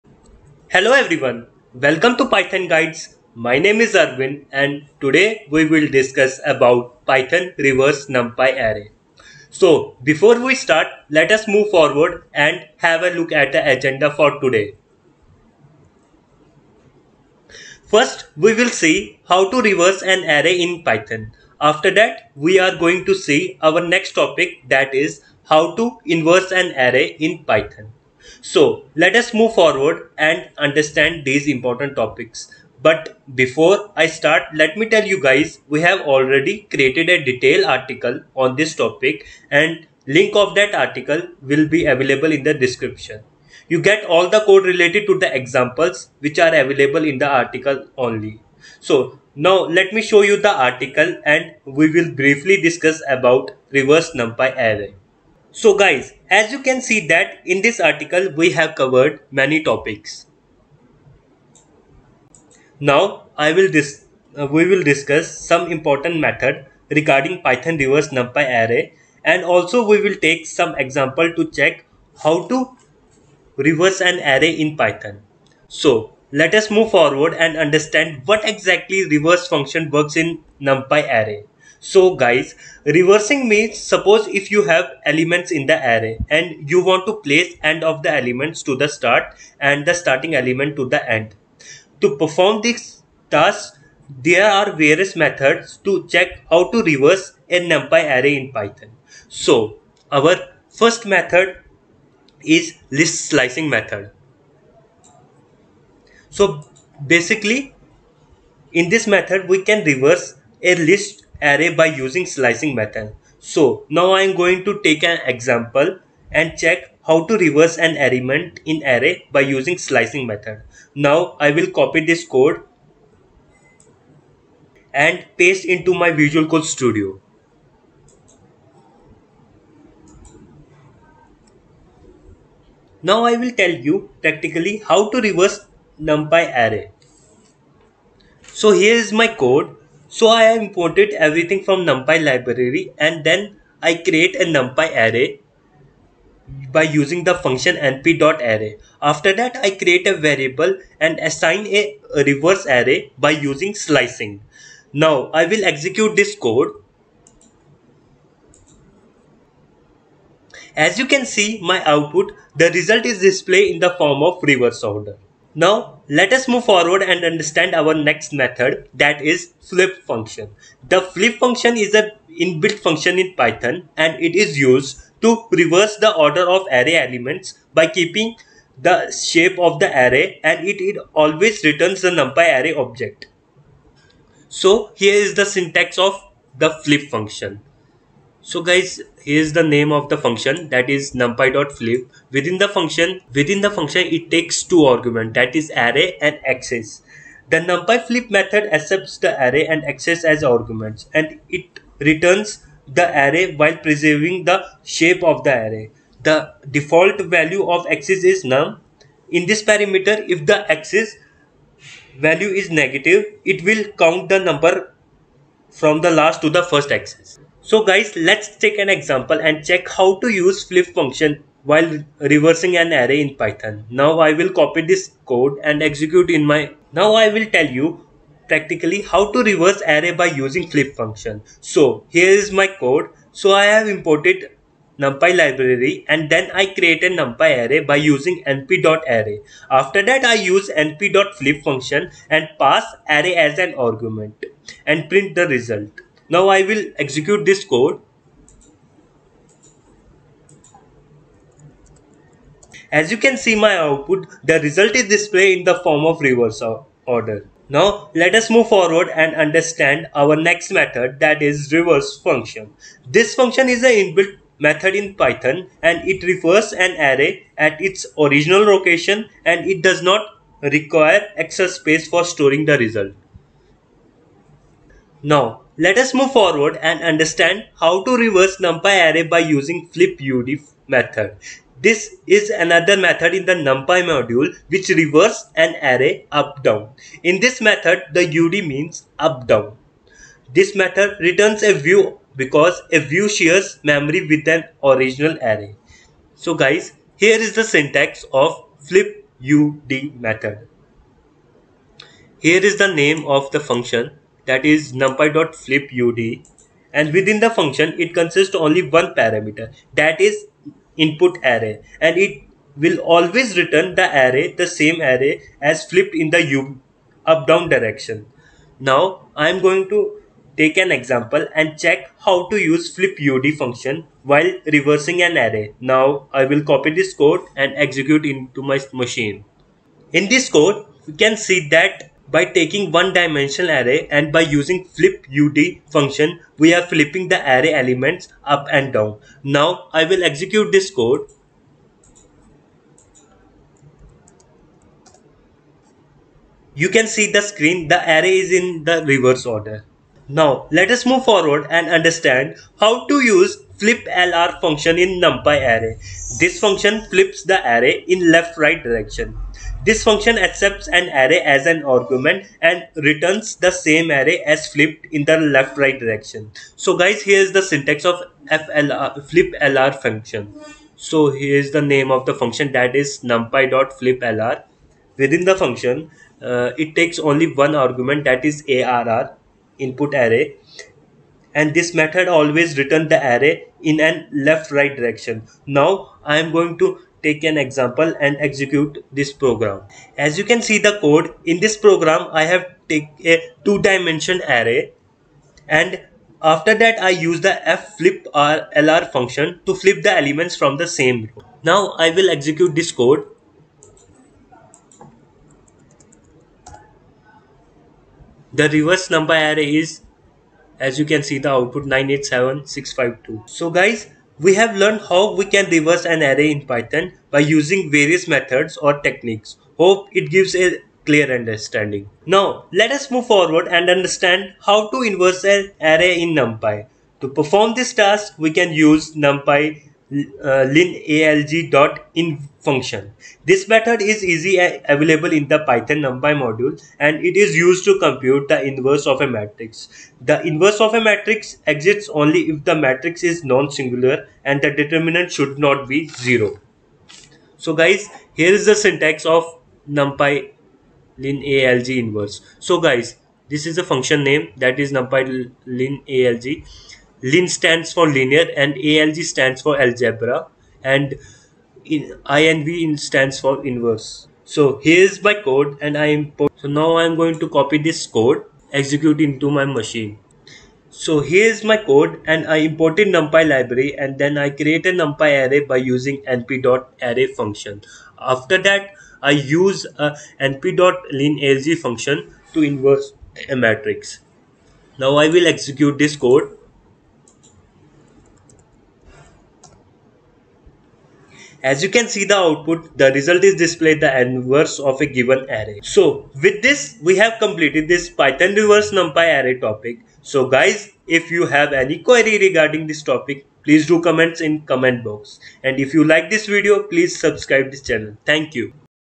How to reverse an array element in NumPy Python
In this Python NumPy video tutorial, I will understand how to reverse NumPy array elements in Python. Here I explain what is reverse numpy array and how to reverse an array in Python.
Additionally, I have covered:
1. How to inverse the array elements in NumPy Python
2. How to reverse NumPy array elements in Python using flipud() method
3. How to reverse NumPy array elements in Python using reverse() method
4. How to reverse NumPy array elements in Python using flip() method

Time Stamp:
0:00- Introduction
0:18- How to reverse an array in Python
1:34- Methods to check How to reverse an array in Python
1:42- List slicing methods
2:14- How to reverse numpy array
3:16- NumPy flip method
3:48- How to reverse array by using flip function
Want to learn machine learning? Check out a complete training course on Python and Machine Learning Training Course